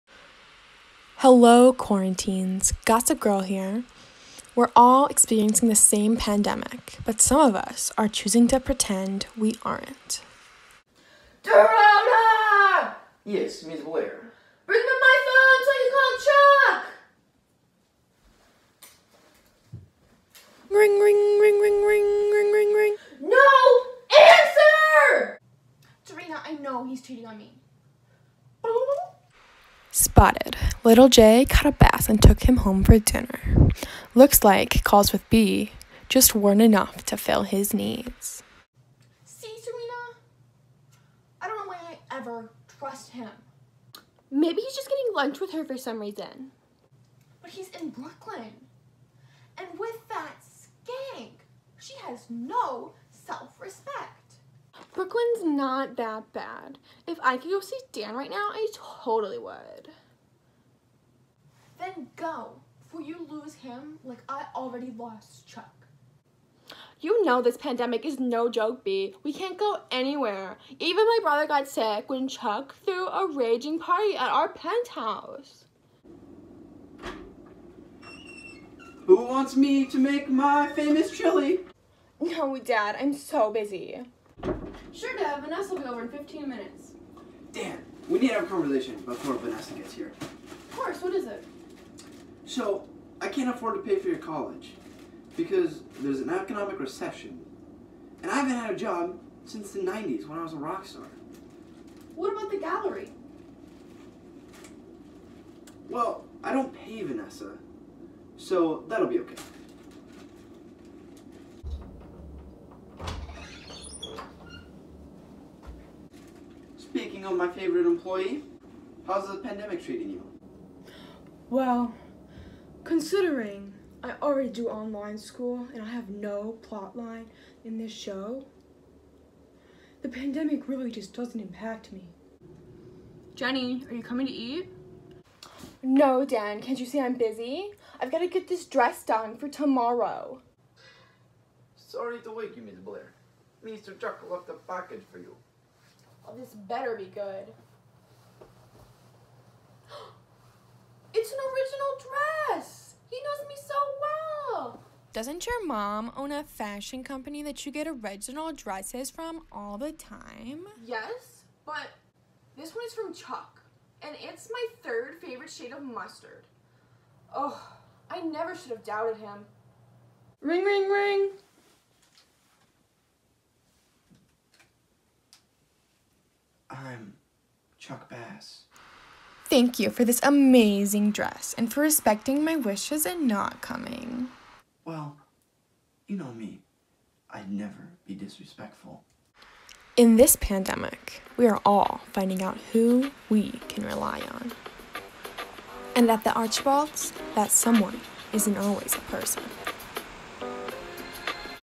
if gossip girl were set during the pandemic
Joe: Rufus, Dorota, Nate, and Chuck
Sophie: Blair, Serena, Dan, Vanessa, and Jenny
Sophie: Writing, Editing, Directing, and Producing
Sophie and Joe: Filming